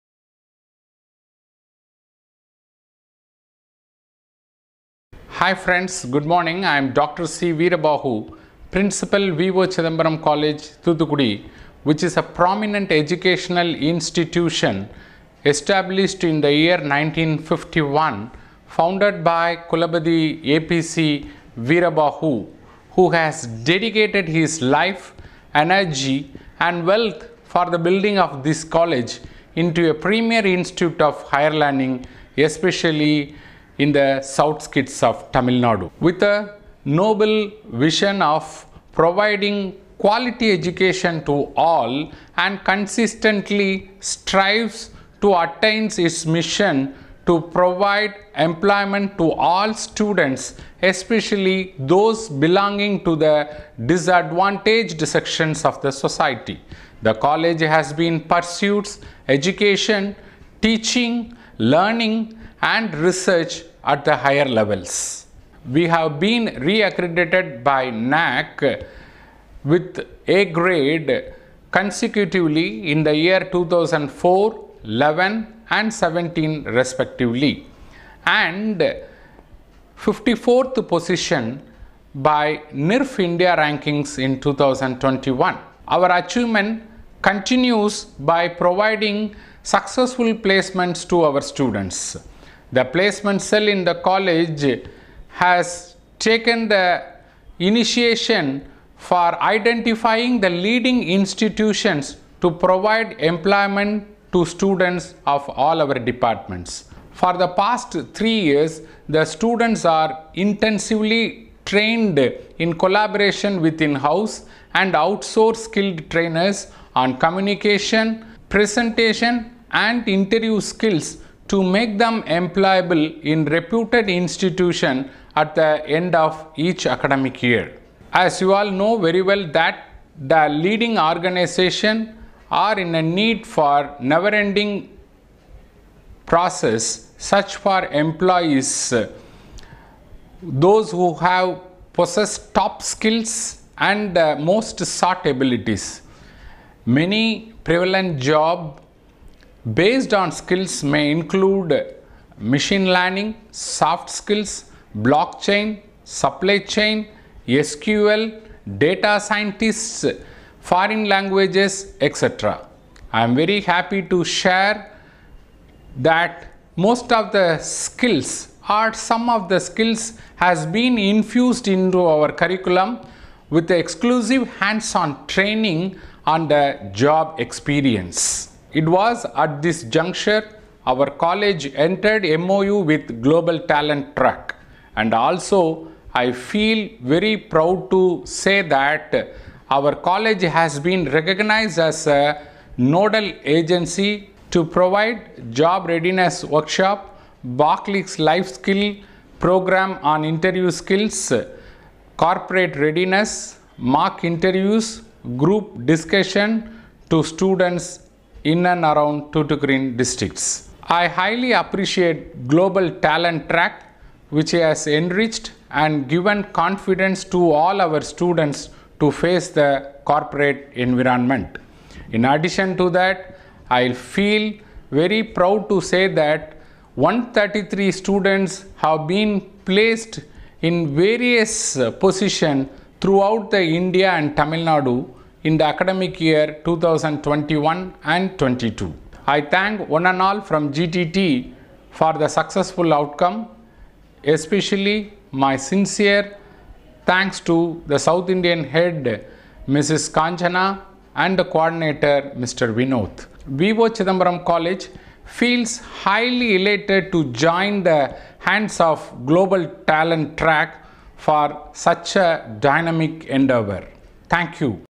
V.O.CHIDAMBARAM COLLEGE, THOOTHUKUDI in association with GLOBAL TALENT TRACK
V.O.CHIDAMBARAM COLLEGE, THOOTHUKUDI in association with GLOBAL TALENT TRACK to provide Skill Based Training for Employment for the Students Community.